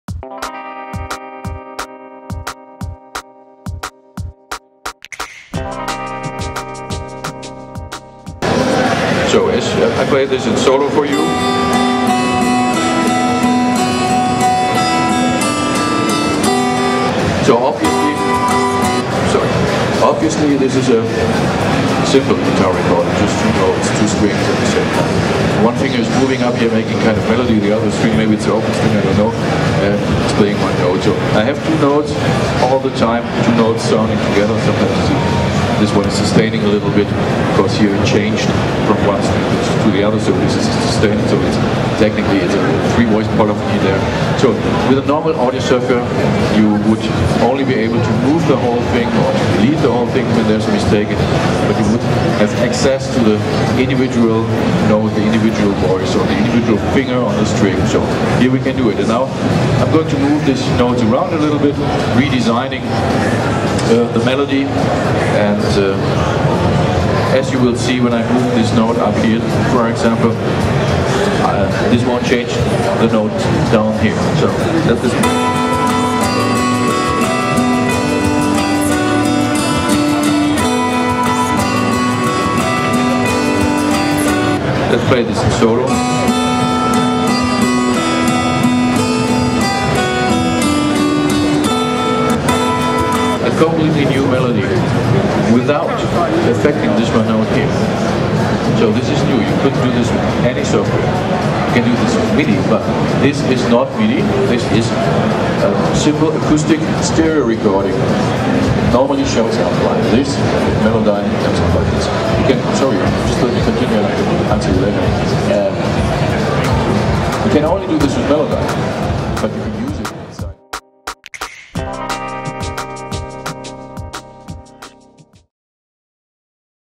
Melodyne Editor - First release with DNA Direct Note Access
Revolution Audio Canada presents Melodyne Editor.  The first Melodyne Plug-in with DNA - Direct Note Access technology. This video is from NAMM 2009 in California.   With the integration of DNA, however, even the harmonies in an audio file can now be seen, accessed and shaped—note by note. Users can quite literally intervene in the chords of their recordings and change individual notes within them. This offers sensational new possibilities for the correction and refashioning of audio.